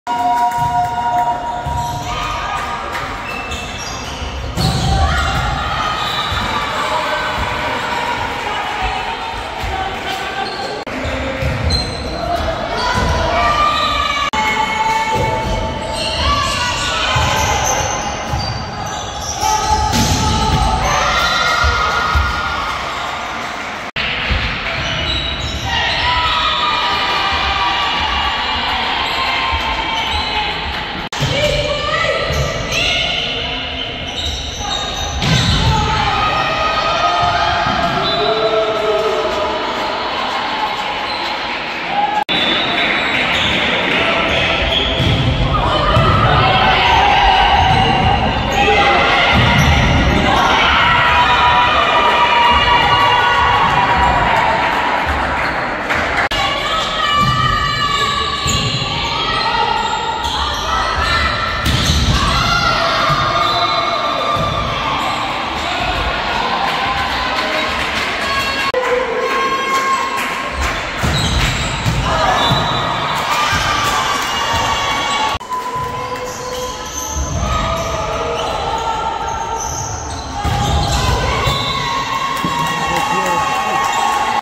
Pamje nga ndeshja Tirana-Vllaznia 3-0.Superliga volejboll femraPallati i sportit “Farie Hoti” Tiranë
Pamje nga ndeshja Tirana-Vllaznia 3-0. Superliga volejboll femra. Pallati i sportit “Farie Hoti” Tiranë